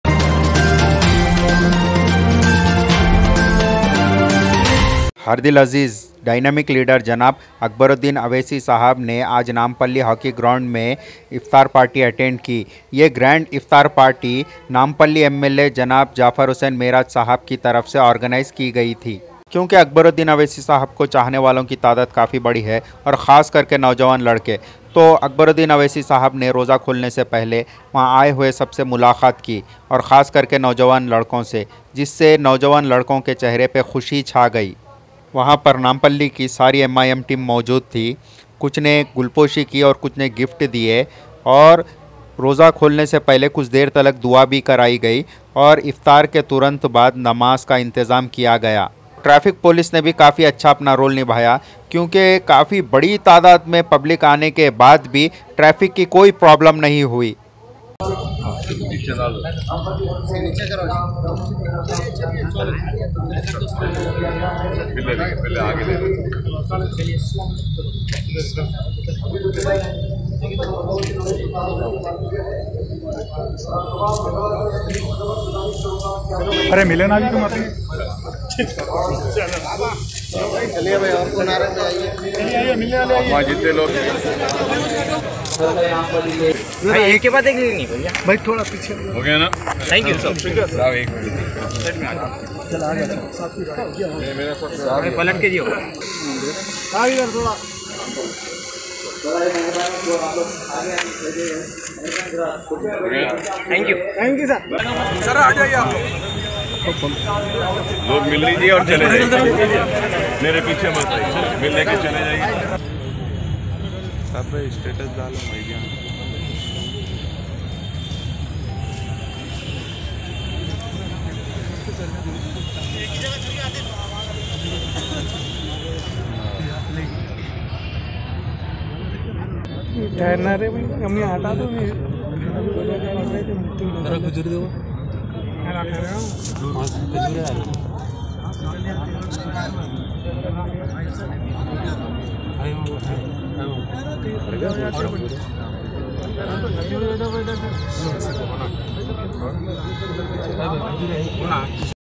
Akber uddin owaisi attend iftar party at Nampally अकबरुद्दीन ओवैसी ने इफ्तार पार्टी अटेंड की।
Akbaruddin Owaisi attended an Iftar party at Nampally Hockey Ground.  It was organized on behalf of MLA Janab Jafar Hussain Meraj Sahab, in which a large number of people came and on Akbaruddin Owaisi Sahib met every young man.  Due to which the young people were very happy and after that, when it was time for Iftar, fasting was broken and there prayers were arranged.  He immediately left after offering Namaz.

अकबरुद्दीन ओवैसी ने नामपल्ली हॉकी ग्राउंड में इफ्तार पार्टी अटेंड की जो एमएलए जनाब जाफर हुसैन मेरा साहब की तरफ से ऑर्गेनाइज की गई थी, जिसमें भारी तादाद में लोग आए और अकबरुद्दीन ओवैसी साहब ने हर एक नौजवान से मुलाकात की। जिस से नौजवान लोग काफी खुश हुए और उसके बाद इफ्तार के वक्त होने पर रोजा खोला और वहीं पर नमाज का इंतजाम किया गया था। नमाज पढ़ने के बाद तुरंत वह निकल गए।
fair news
fair news
FAIR NEWS